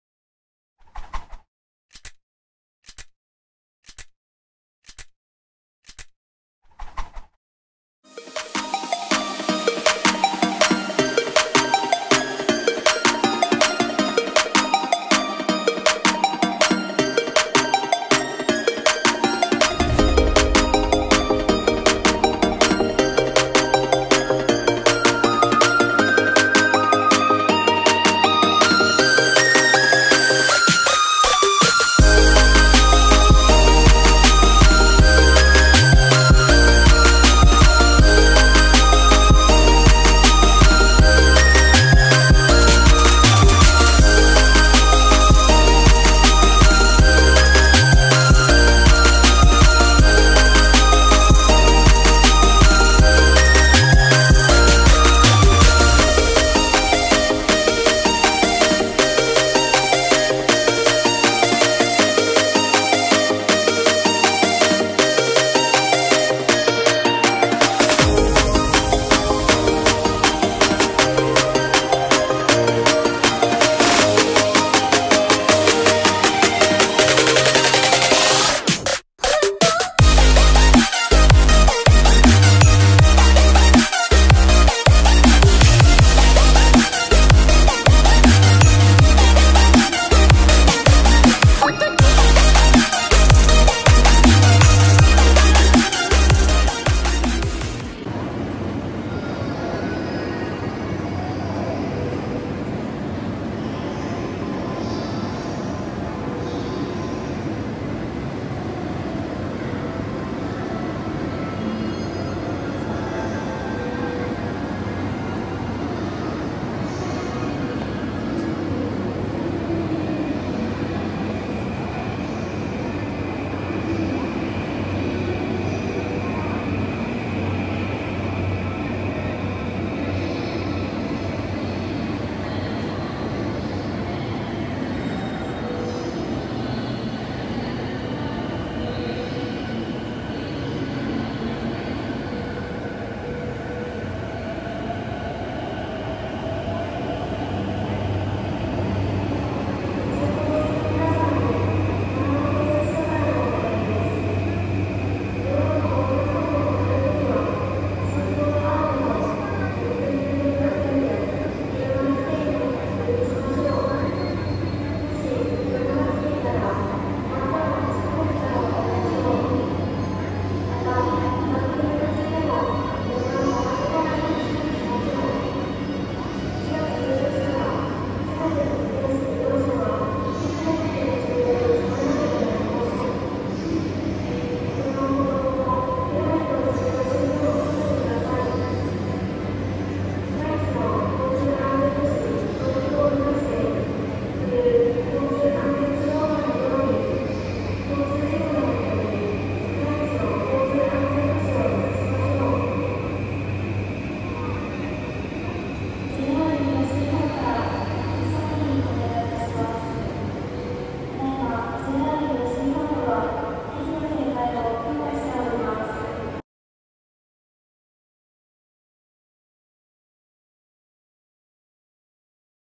【高輪ゲートウェイ駅】新しい駅の高輪ゲートウェイ駅にお出かけvlog
家族で高輪ゲートウェイ駅に行ってきました。綺麗な駅で山手線、京浜東北線はもちろん、近くを通る新幹線や特急ときわなども見られて、子供たちも大満足！